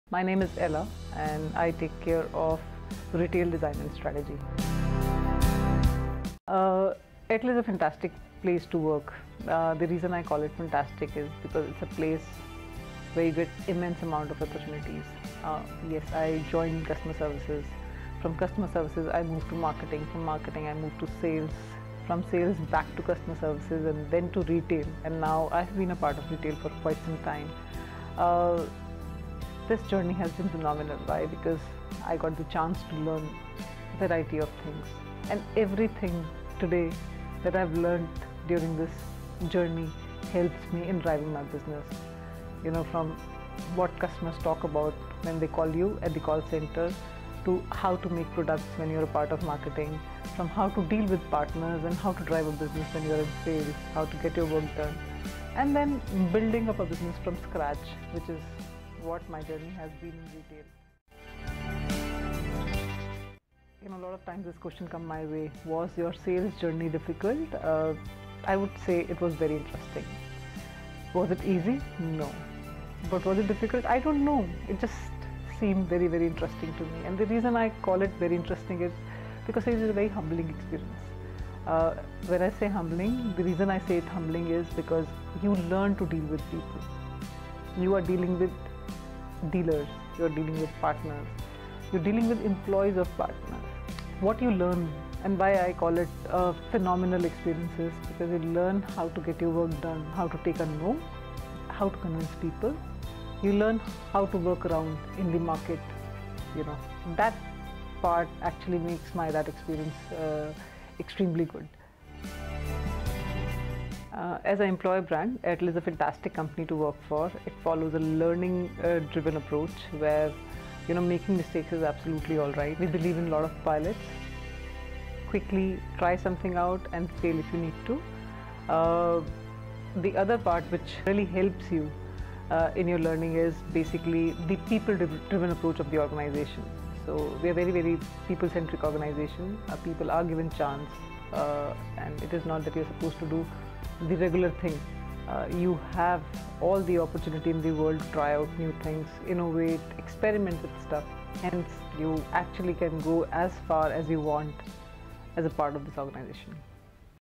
Ella Garg, Vice President - Retail Design
Listen to Ella Garg, VP - Retail Design talk about what Airtel means to her, the journey at Airtel and most enriching experiences across various functions of Sales, Customer Experience, Marketing and Retail